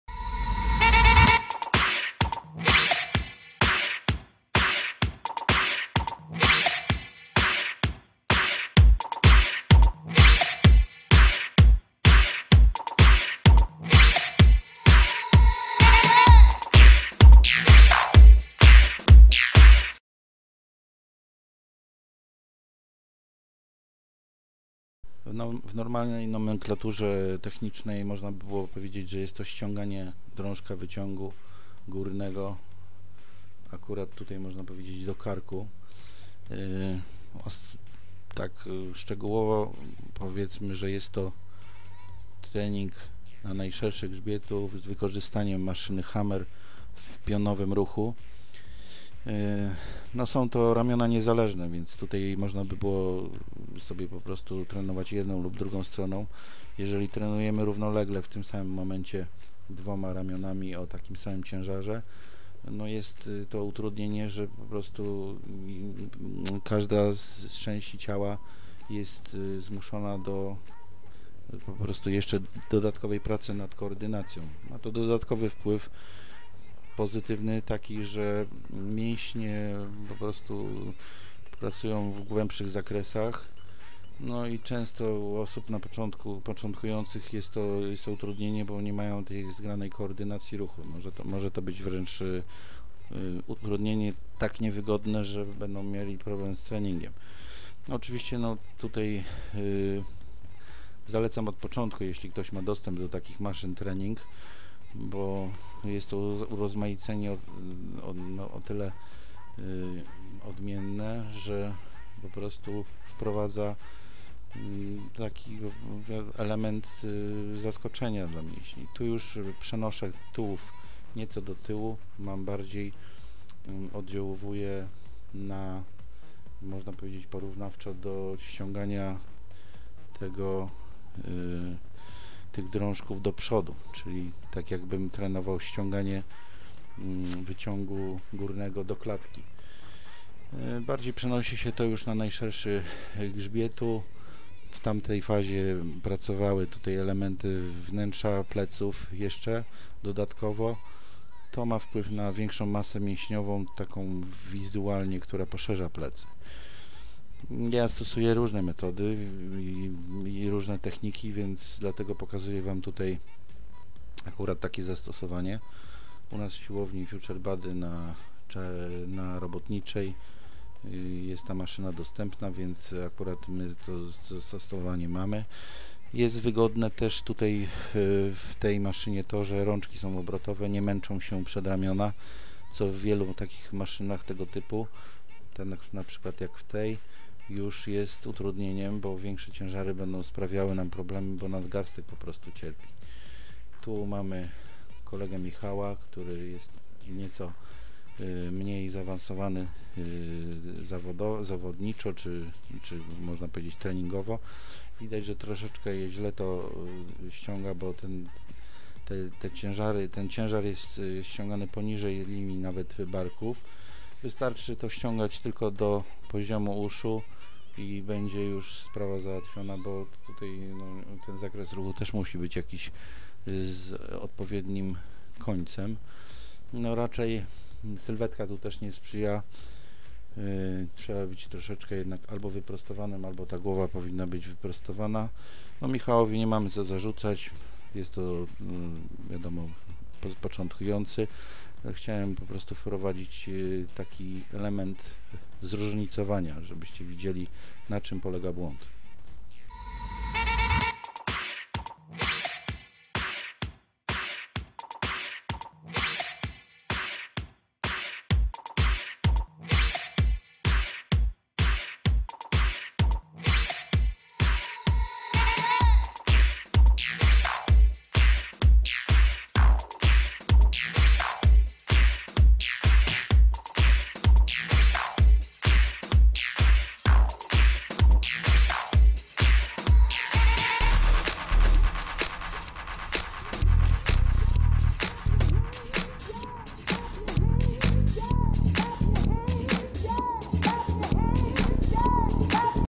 Poprawna technika wykonywania ćwiczeń. Plecy - Vol.13
Imitacja ściągania drążka wyciągu pionowego do karku
w maszynie typu Hammer